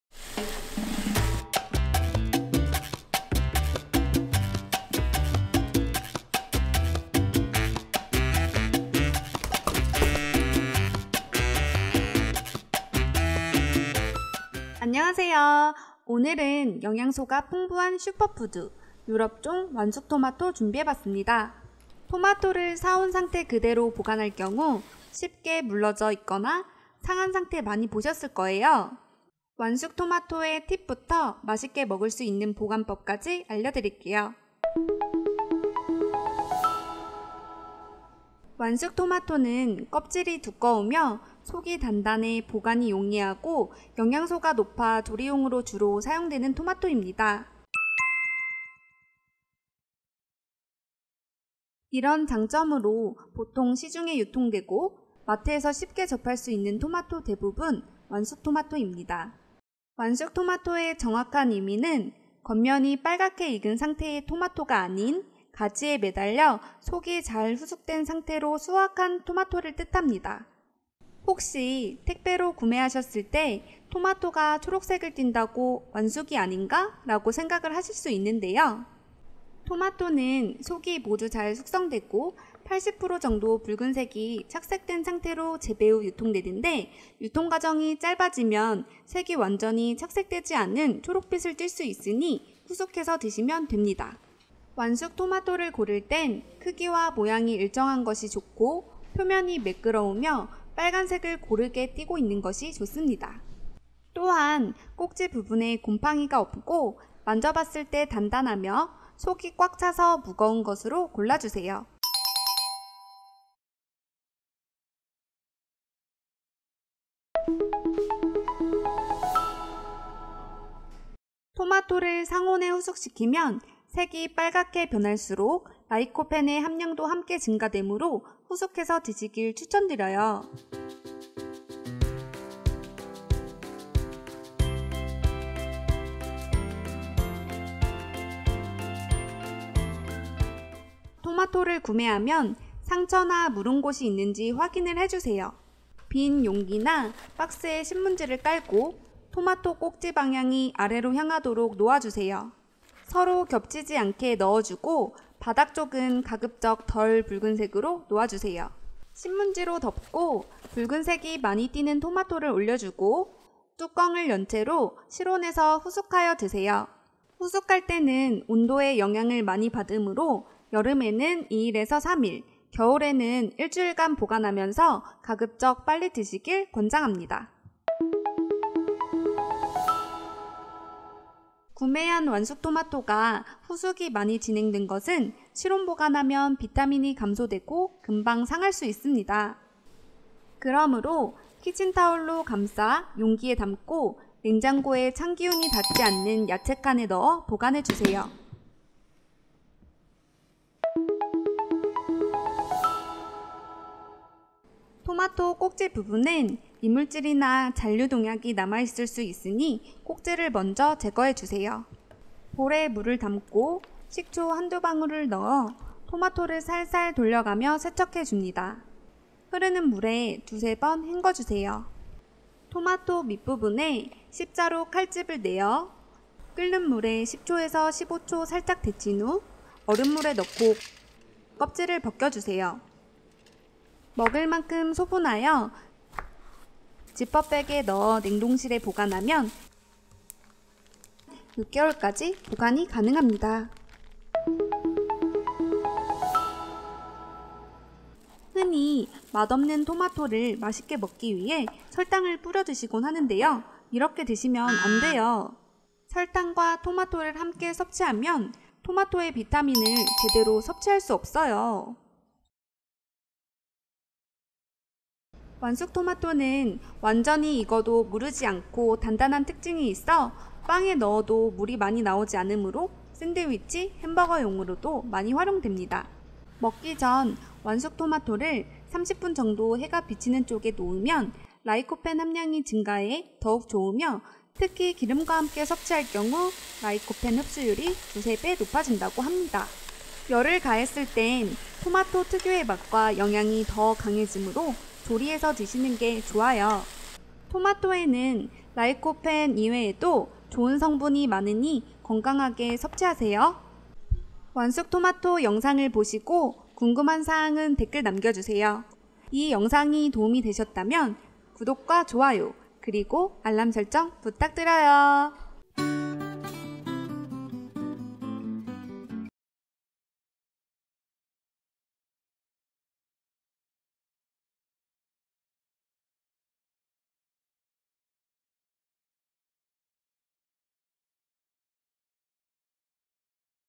우리가 몰랐던 사실 토마토 보관법 영양소 200% 섭취하기
오늘은 영양소가 풍부한 슈퍼푸드 유럽종 완숙토마토 보관법 준비해 봤습니다.

시중에 유통되고 마트에서 쉽게 접할 수 있는 토마토 대부분 완숙  토마토 입니다.  예전에는 단맛이 있는 찰토마토가 많이 있었으나 저장성이 떨어져 지금은 찾아보기 어렵습니다

완숙 토마토의 정확한 의미는 겉면이 빨갛게 익은 상태의 토마토가 아닌 가지에 매달려 속이 잘 후숙된 상태로 수확한 토마토를 뜻합니다.

토마토를 상온에 후숙시키면 색이 빨갛게 변할수록 라이코펜의 함량도 함께 증가 하므로 후숙해서 드시길 추천해 드려요.

흔히 맛없는 토마토를 맛있게 먹기 위해 설탕을 뿌려 드시고는 하는데요 이렇게 드시면 안 돼요!

자세한 내용은 영상 참고해주세요.

▶ 혹시라도 잘못된 정보나 궁금하신 사항이 있으시면 댓글 남겨주세요!!